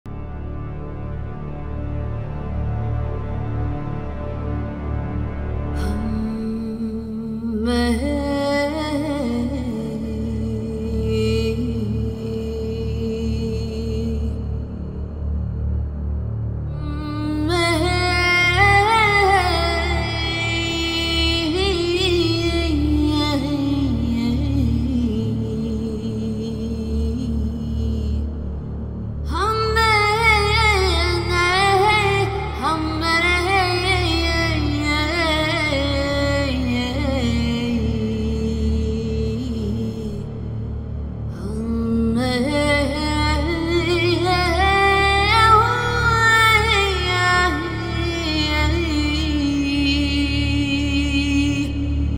Unrest in Palestine continues to worsen.
Palestine needs your support as unrest continues to get worse each day! The team from MATW Project are on ground providing medical aid, food & water.l and more.